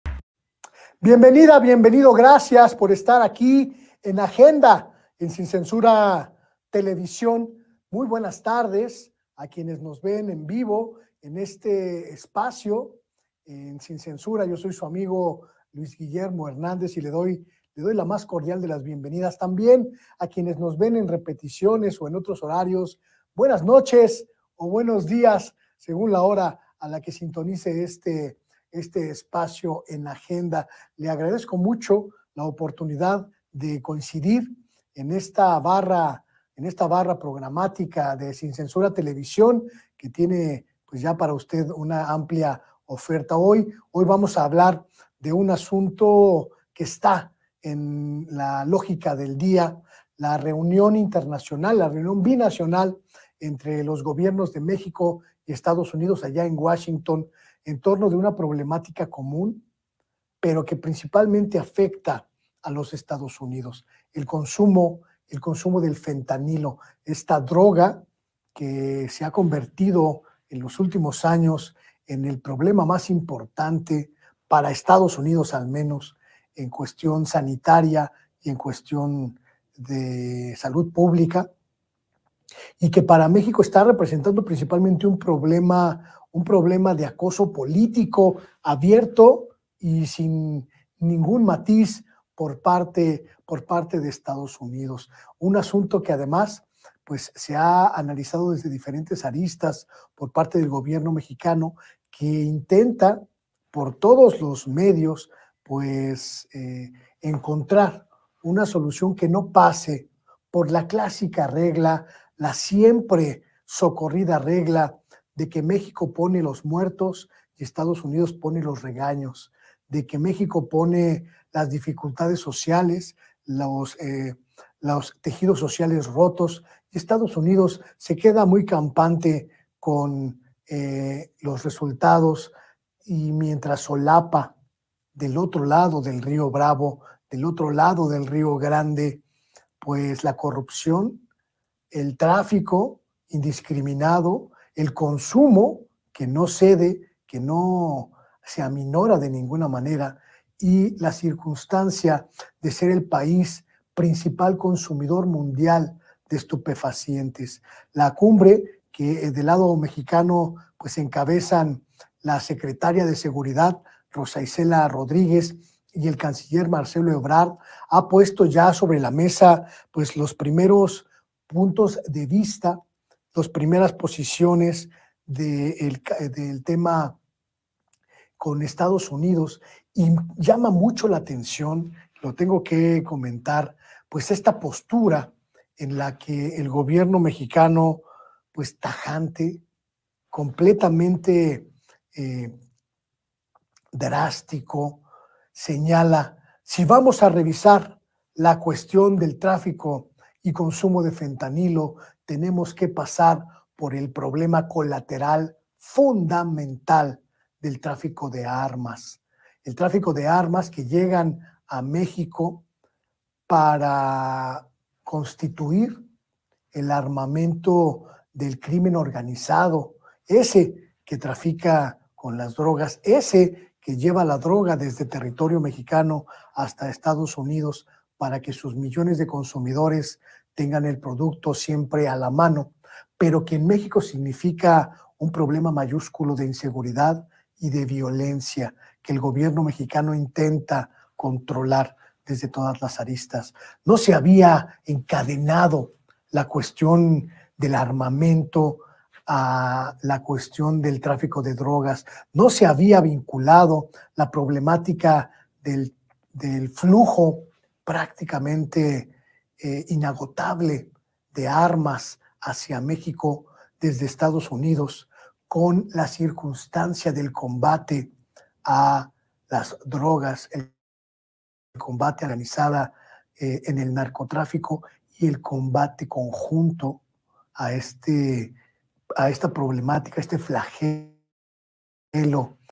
MÉXICO TAJANTE DICE A EEUU QUE PARA ANALIZAR TRÁFICO DE F3NT4N1L0 TAMBIÉN REVISEN TRÁFICO DE ARMAS
El periodista independiente Luis Guillermo Hernández reflexiona sobre el encuentro bilateral México-Estados Unidos para combatir el tráfico de precursores químicos, drogas sintéticas y armas.